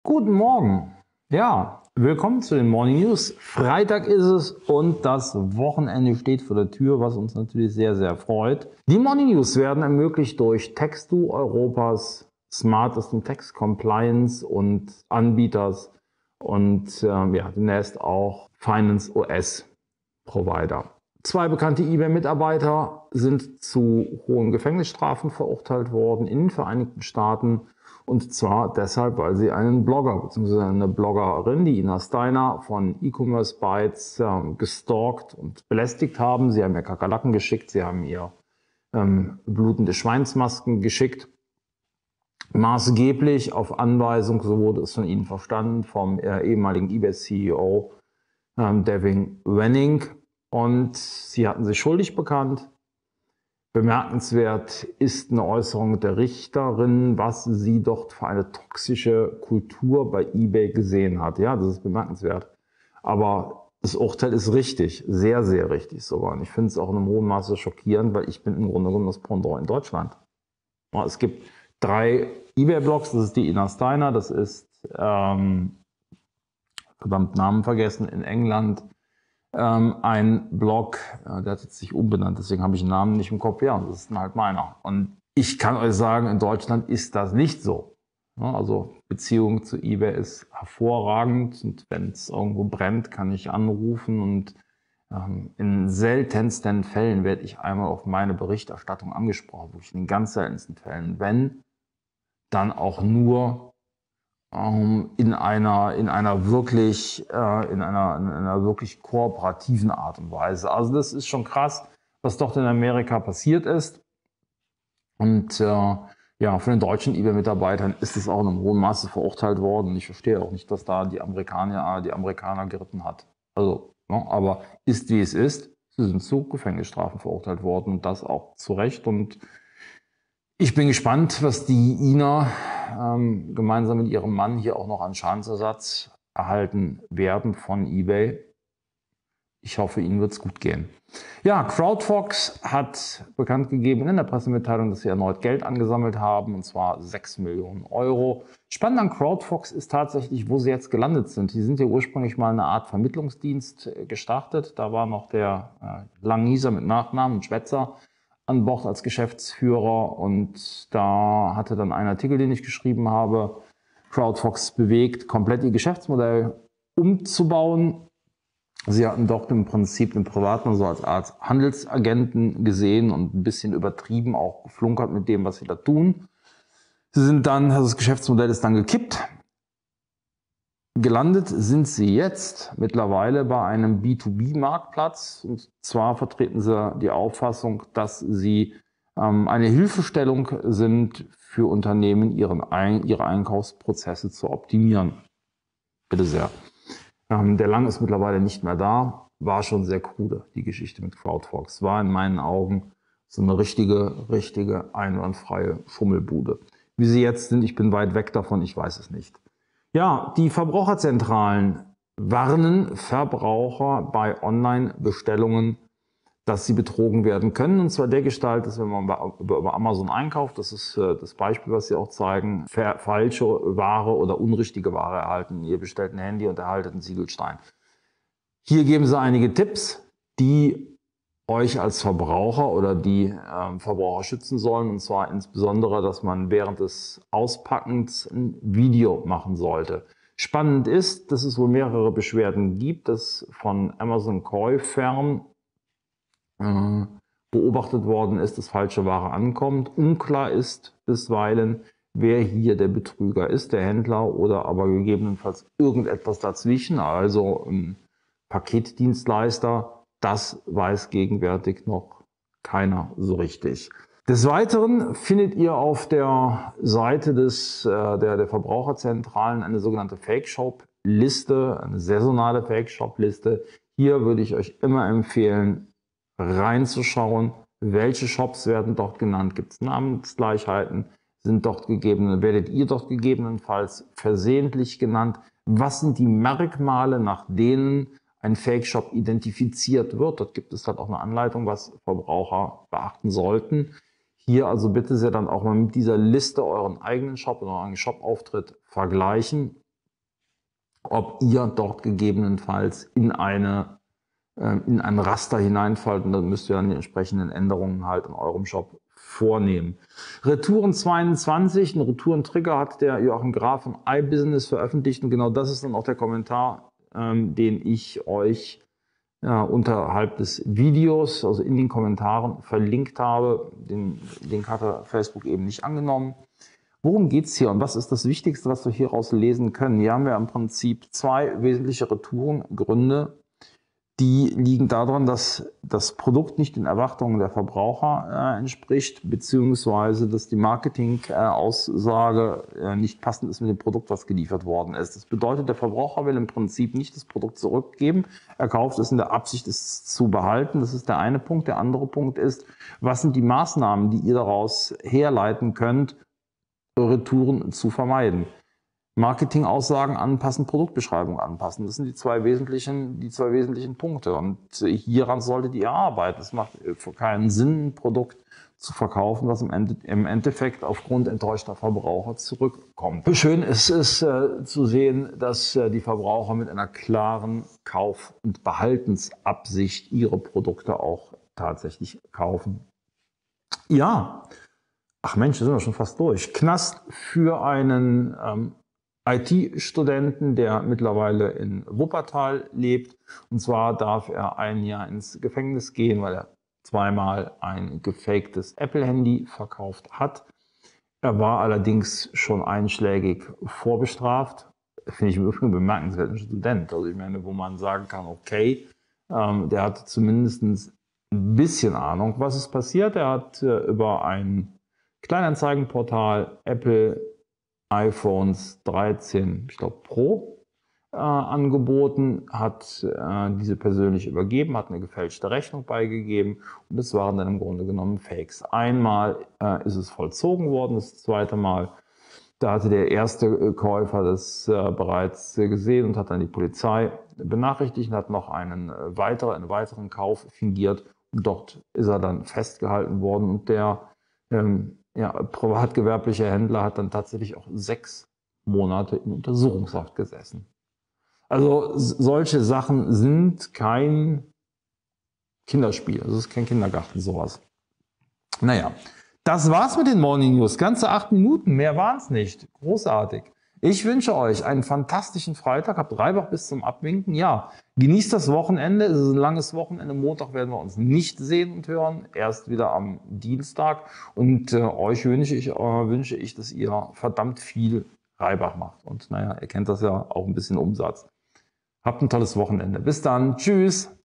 Blutende Schweinsmasken versendet: eBay Mitarbeiter verurteilt!
✅Die News werden durch Taxdoo ermöglicht✅

🎯Live Morning News 30. Sep. 2022 | Taxdoo Facts | | eBay Mitarbeiter im Knast | crowdfox | vbz rät Video machen | Fakeshop Liste | Retouren 2022 | Knast für Handy Verkäufer

Zeitstempel:
00:00 eBay Mitarbeiter im Knast
02:20 crowdfox
03:29 vbz rät Video machen
04:24 Fakeshop Liste
05:10 Retouren 2022
06:26 Knast für Handy Verkäufer

Um immer auf dem Laufenden zu bleiben:
Marktplatz & E-Commerce News – kritisch, ehrlich & direkt

Die Facebookgruppe für eBay Amazon FBA Online-Handel E-Commerce Abmahnung Alibaba